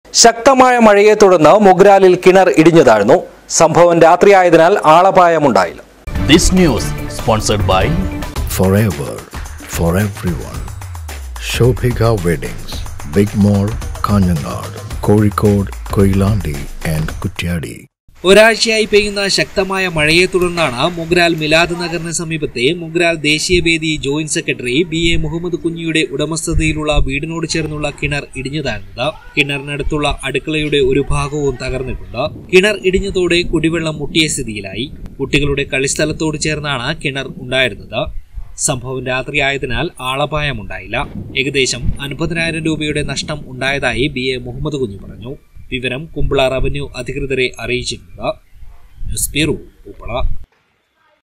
ശക്തമായ മഴയെ തുടര്‍ന്ന് മൊഗ്രാലില്‍ കിണര്‍ ഇടിഞ്ഞു
ശക്തമായ മഴയെ തുടര്‍ന്ന് മൊഗ്രാലില്‍ കിണര്‍ ഇടിഞ്ഞു താഴ്ന്നു. സംഭവം രാത്രിയായതിനാല്‍ ആളപായമുണ്ടായില്ല.